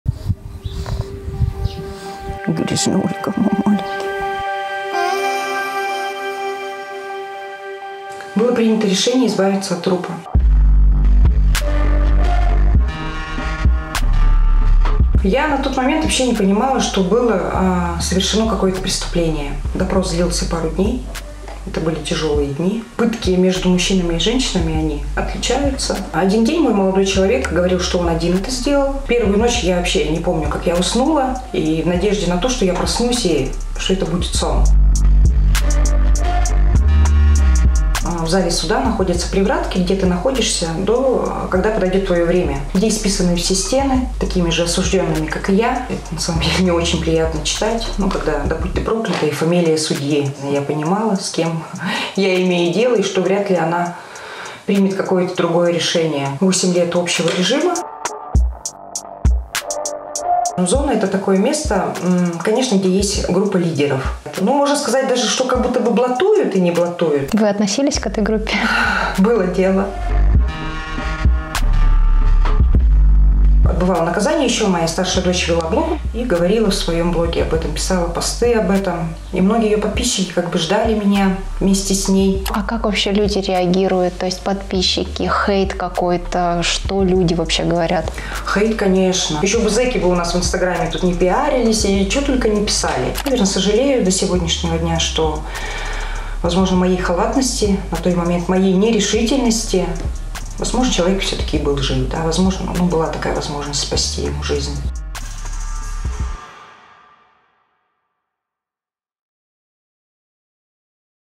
«Надя зэчка»: о тюрьме, онкологии, хейте и мужчинах. Анонс интервью
Наш следующий сюжет о Надежде Лыгдыповой. На первый взгляд, обычная женщина средних лет - мама, бабушка, мастерица на все руки. Но есть одно «но». Бывшая заключенная поделилась своей историей, рассказала о тюрьме, болезни, блоге, хейте и мужчинах. Совсем скоро вы увидите полный выпуск, а пока анонс.